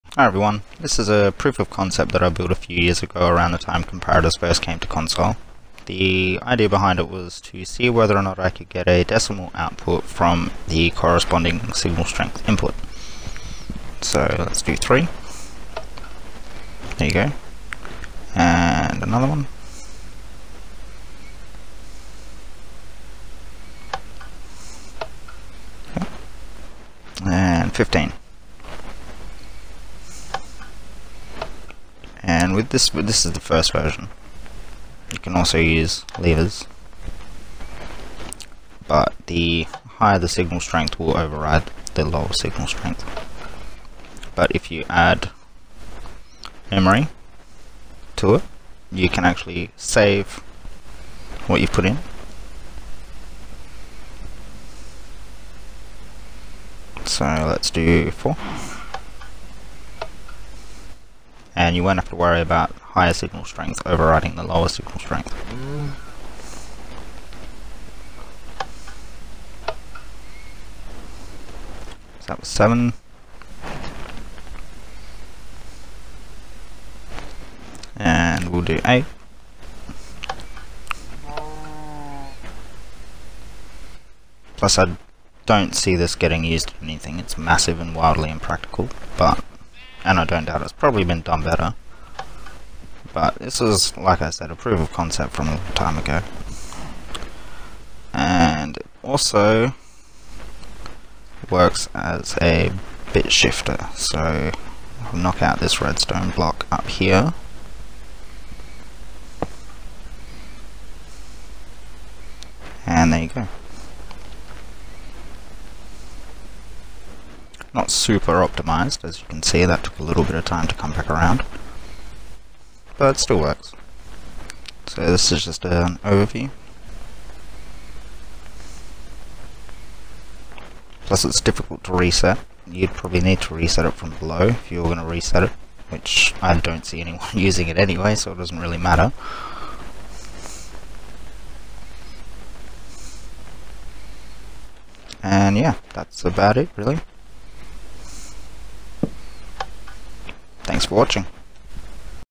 Signal strength to decimal
This take a signal strength input and outputs it to a lamp in the corresponding decimal position.